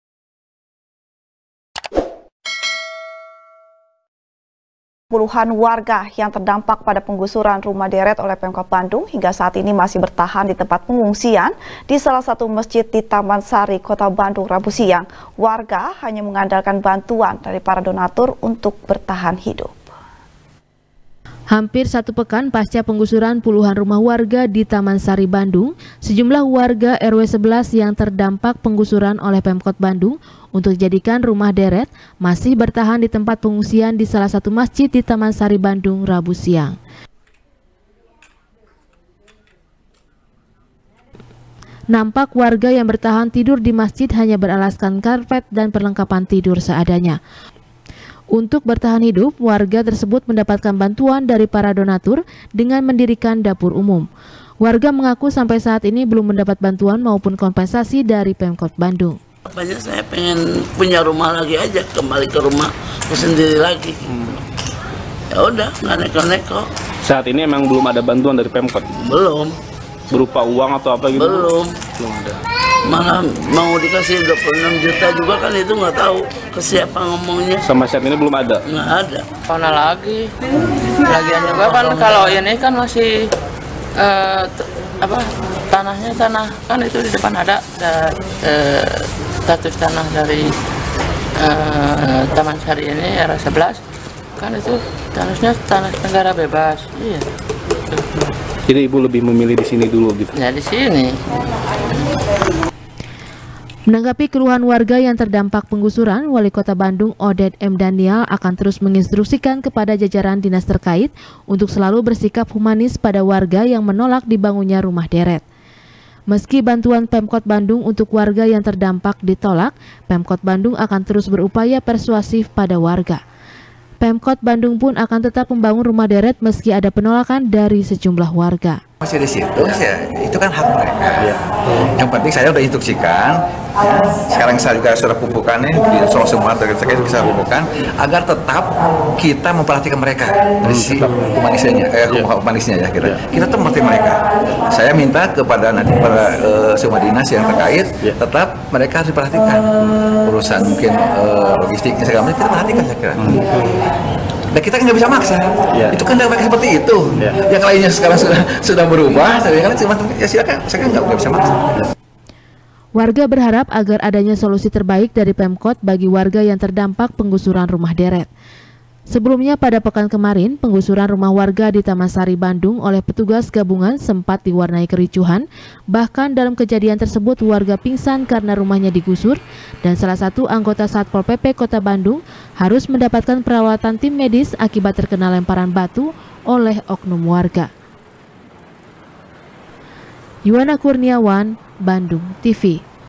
Sebagian Warga Taman Sari Masih Bertahan Di Mesjid | SBR | BANDUNGTV
SBR | BANDUNGTV | FARHAT MUMTAZ | Puluhan  Warga Yang Terdampak Pada Pengusuran Rumah Deret Oleh Pemkot Bandung, Hingga Saat Ini Masih Bertahan Di Tempat Pengungsian Di Salah Satu Masjid Di Taman Sari Kota Bandung Jawa Barat, Rabu Siang. Warga Hanya Mengandalkan Bantuan Dari Para Donatur, Untuk Bertahan Hidup.